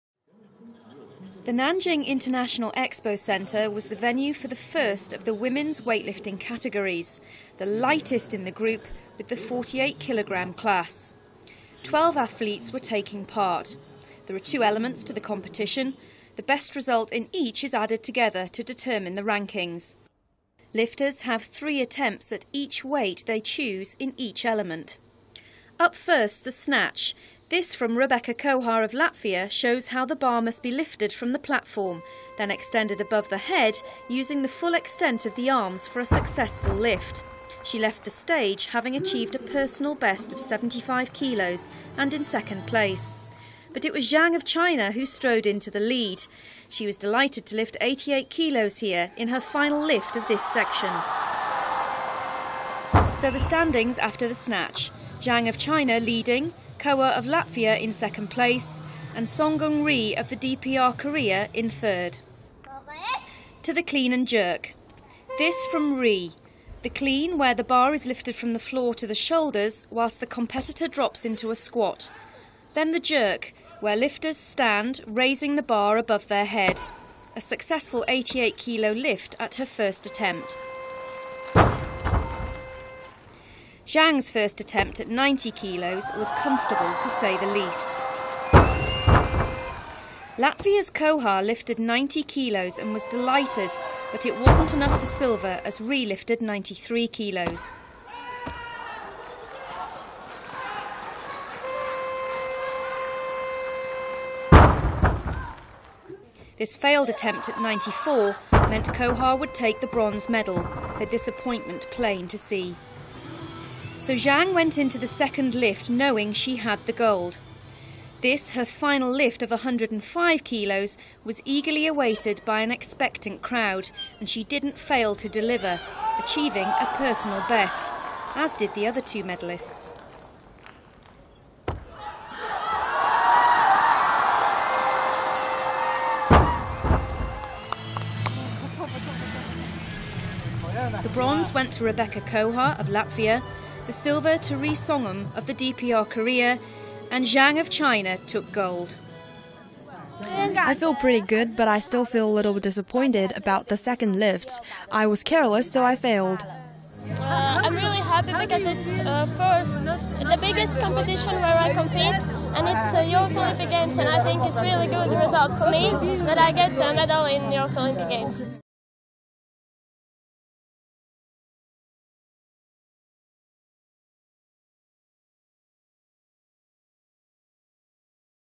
Svarcēlāja Rebeka Koha II Jaunatnes Olimpiskajās spēlēs izcīna bronzu
Lielisku panākumu sasniedza Latvijas svarcēlāja Rebeka Koha, kura izcīnīja bronzas medaļu svara kategorijā līdz 48 kilogramiem.

Mūsu vienīgā svarcēlāja šajās Olimpiskajās spēlēs Rebeka Koha startēja svara kategorijā līdz 48 kilogramiem. Trenera Eduarda Andruškēviča audzēkne ikdienā trenējās Ventspilī, bet savā karjerā guvusi nozīmīgus starptautiskus panākumus.

Raušanā Rebekas sākuma svars bija 71 kilograms, ko mūsu sportiste arī sekmīgi pacēla. Turpinājumā latviete tika galā arī ar 73 un 75 kilogramiem. Labāk par Kohu šajā svarcelšanas veidā veicās tikai mājiniecei Jiangai Huihuai, kura trešajā mēģinājumā pacēla 88 kilogramus. Trešajā vietā tikmēr ierindojās ziemeļkorejiete Ri Sonngumai, kas no Latvijas pārstāves atpalika par trijiem kilogramiem. Citas konkurences bija vēl krietni tālāk.

Viss izšķīrās grūšanā. Rebekas Kohas un viņas trenera pieteiktais sākuma svars bija 86 kilogrami, ko arī mūsu sportiste pievarēja. Rebeka pacēla arī 93 kilogramus, stabili atrodoties labāko trijniekā. Pēdējā piegājienā latviete centās pacelt 94 kilogramus, tomēr šis svars palika nepievarēts. Ventspilnieces kopējais rezultāts summā bija 165 kilogrami. Tik pat daudz summā pacēla arī jau pieminētā Ziemeļkorejas pārstāve. Pateicoties nedaudz mazākam pašas svaram, sudraba medaļu ieguva Sonnguma, pārliecinoši zelts tika ķīnietei Jiangai Huihuai. Tikmēr mūsu sportiste Rebeka Koha izcīnīja bronzas medaļu, kas Latvijas sporta vēsturē vasaras Jaunatnes Olimpiskajās spēlēs ir tikai otrā godalga.